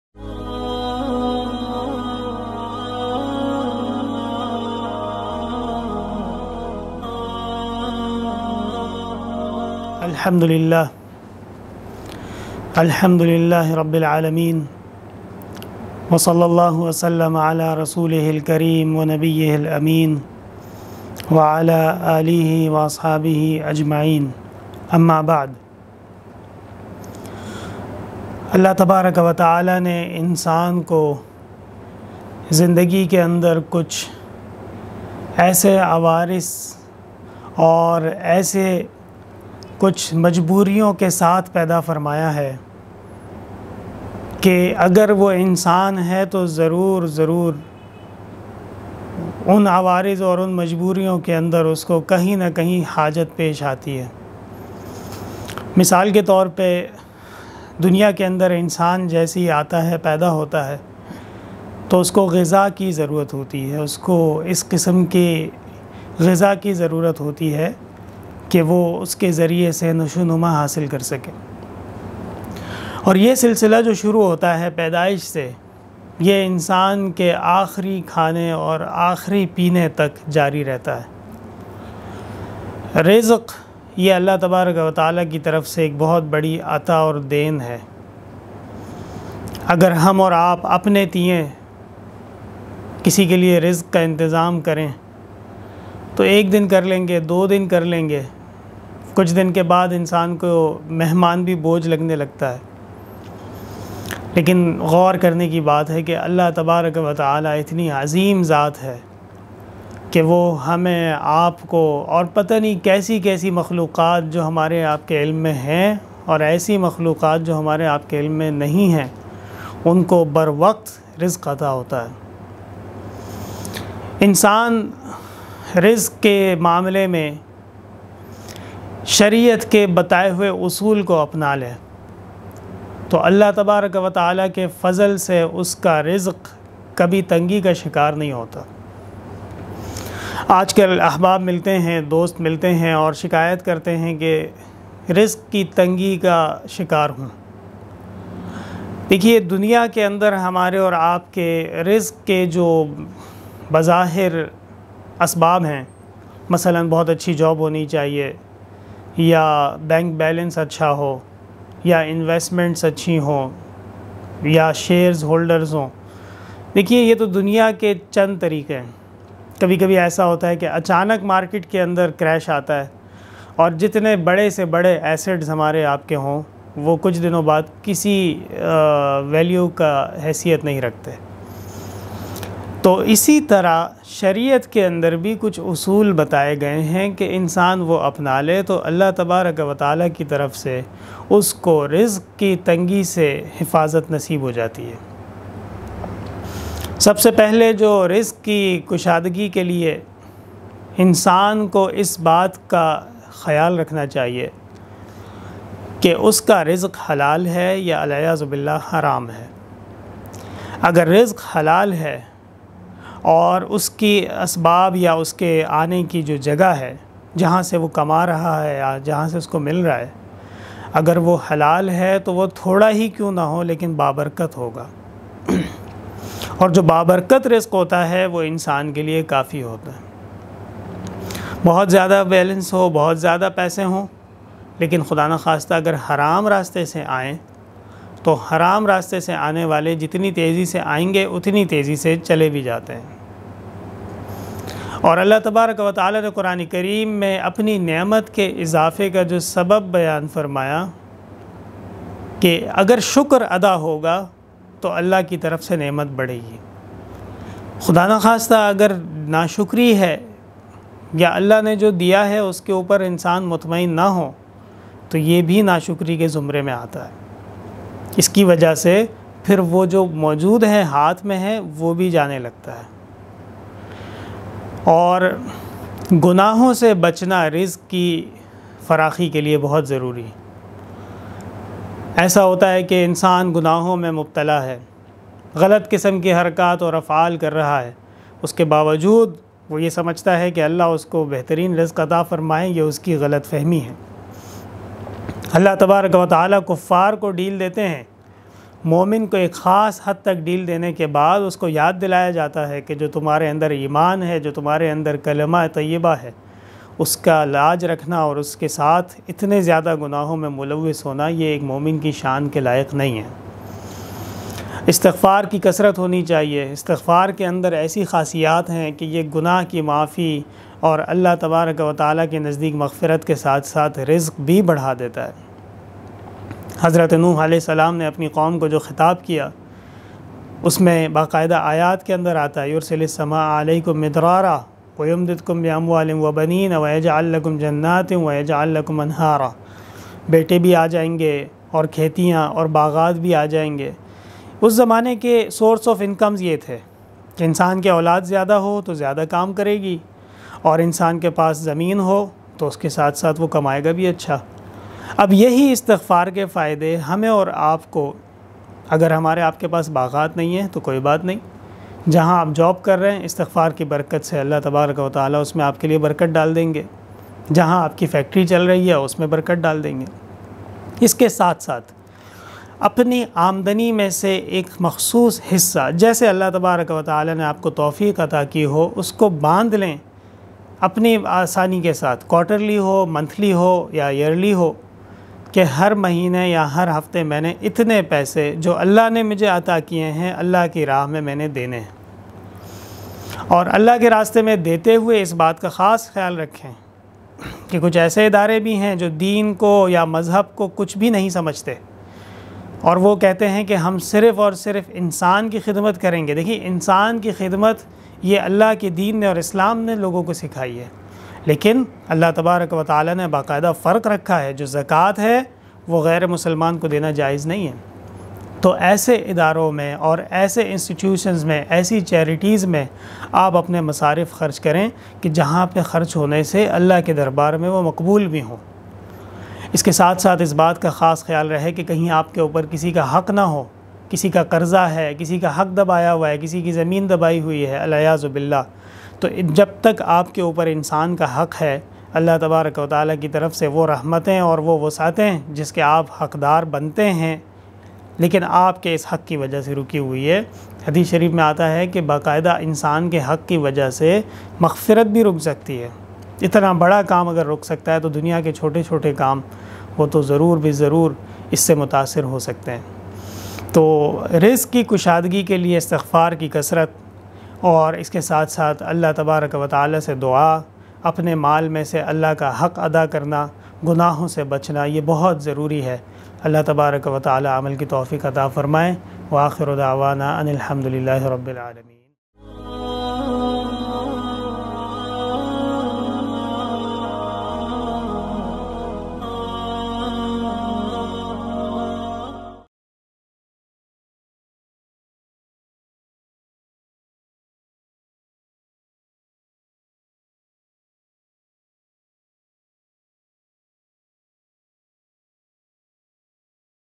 Dars # 018 | Narrowness in the livelihood| Molana Muhammad Anwar Shah
Bayan by: MolanaMohammad Anwar Shah